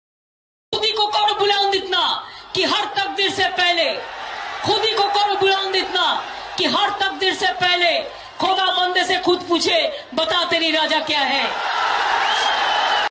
Mamta Bannerjee narrating Hz Dr Allama Iqbal (ra) sacred kalam. |Zinda rood| انقلاب
This is a Laghnat on All those Who divided pakistan on Linguistic Basis, specially those who created Hatred among Bengalis based on Urdu being set us National Unifying Language
They can wake up from their graves to see Mamta Bannerjee narrating Allama Iqbal saab in URDU!